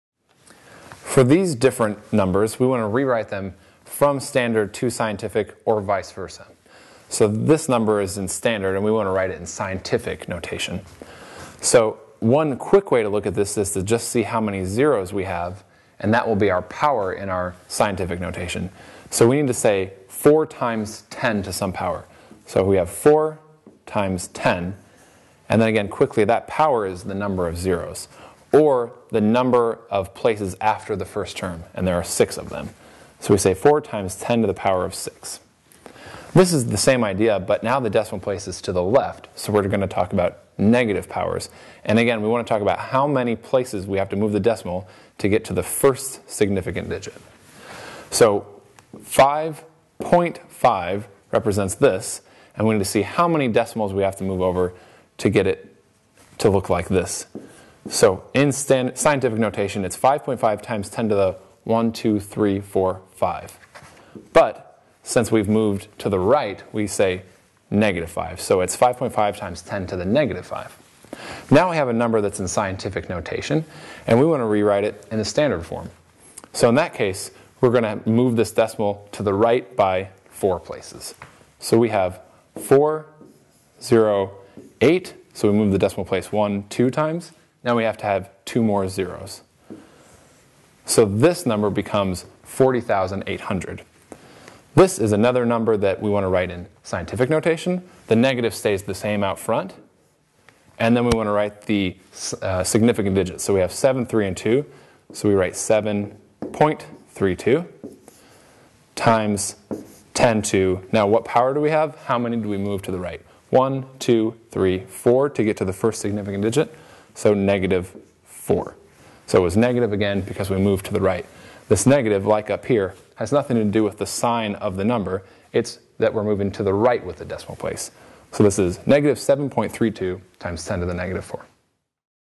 Scientific Notation - Conversion Made Easy
Learn how to quickly convert numbers from standard to scientific notation and vice versa. Follow along as we walk you through examples step-by-step.